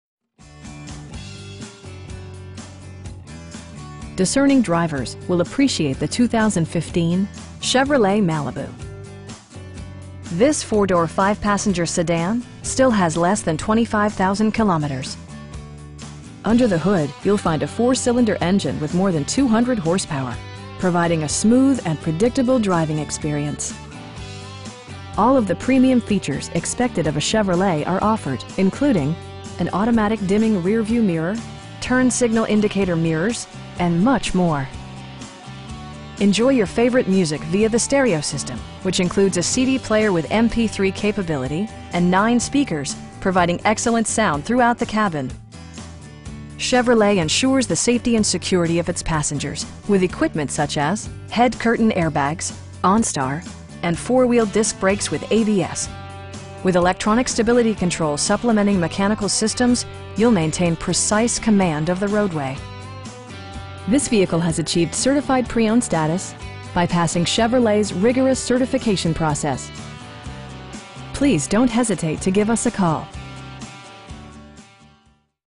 2015 Chevrolet Malibu 2LZ Heated Seats & sunroof
2595 Barnett Highway.

Discerning drivers will appreciate the 2015 Chevrolet Malibu. This 4 door, 5 passenger sedan still has less than 25,000 kilometers! Under the hood you'll find a 4 cylinder engine with more than 200 horsepower, providing a smooth and predictable driving experience. All of the premium features expected of a Chevrolet are offered, including: leather upholstery, front and rear reading lights, power front seats, an automatic dimming rear-view mirror, turn signal indicator mirrors, lane departure warning, and much more. With high intensity discharge headlights illuminating your path, you'll always appreciate maximum visibility. Chevrolet ensures the safety and security of its passengers with equipment such as: head curtain airbags, front and rear SIDE impact airbags, traction control, brake assist, a panic alarm, onStar, and 4 wheel disc brakes with ABS. With electronic stability control supplementing mechanical systems, you'll maintain precise command of the roadway. This vehicle has achieved Certified Pre-Owned status, by passing Chevrolet's rigorous certification process. We pride ourselves in the quality that we offer on all of our vehicles. Please don't hesitate to give us a call.